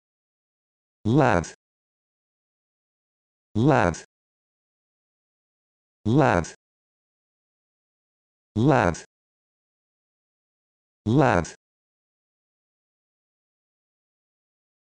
How to Pronounce Lethe
This video shows you how to pronounce Lethe